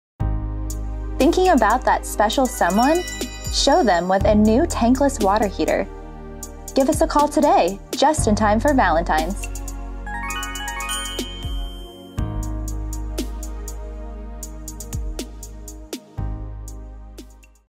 Happy Valentine's Day from Budget!
Thinking about that special someone this Valentine's Day? Show them with a call to Budget and a brand new tankless water heater!